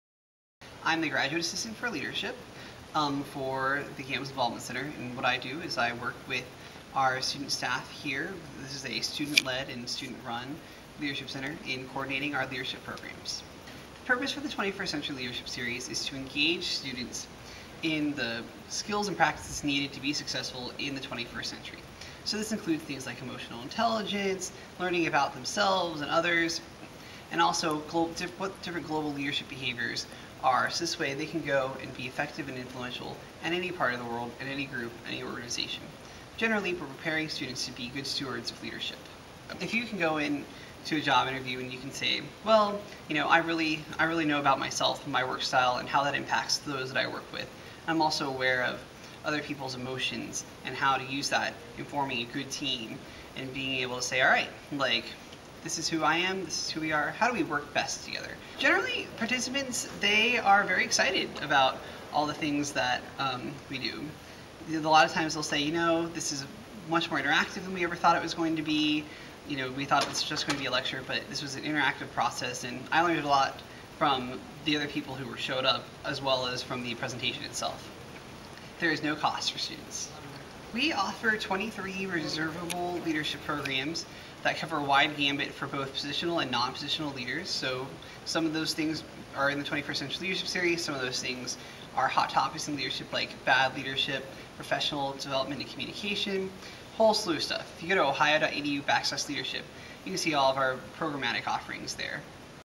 Amanda J. Cunningham Leadership Center
Valuable offerings and benefits of the Amanda J. Cunningham Leadership Center at Ohio University in Athens,Ohio